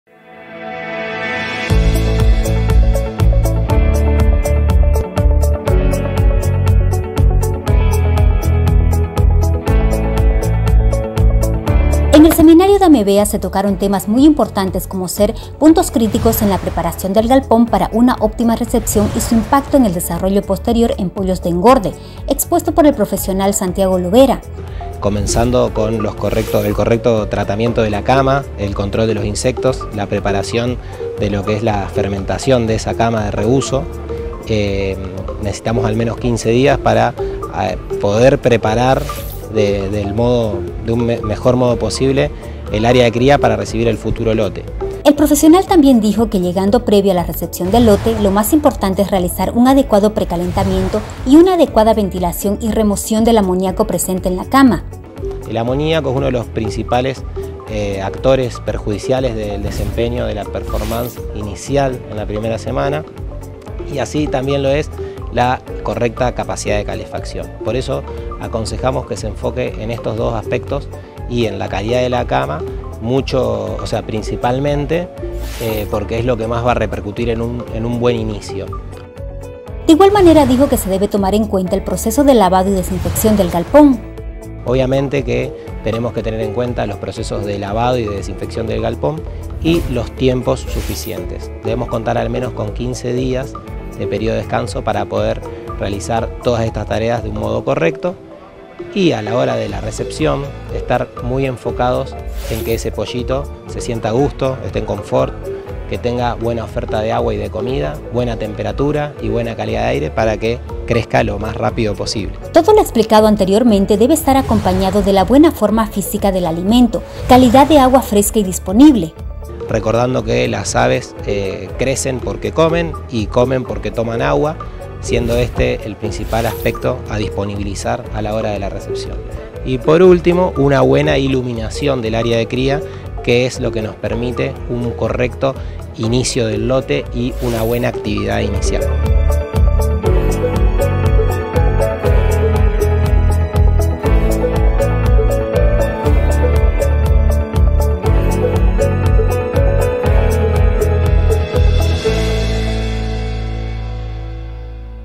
PUBLIAGRO: RECEPCIÓN DE LOS POLLITOS BB SANTIAGO LOVERA
PubliagroInforma | El impacto de una buena recepción de los pollitos BB, tener un galpón preparado para ese fin, pero sobre todo controlar todos los aspectos para que el Pollito BB se adecue correctamente, fue los datos que expuso Santiago Lovera.

¡Saludos productivos! 🇧🇴

Para más detalles, mira el video 🔽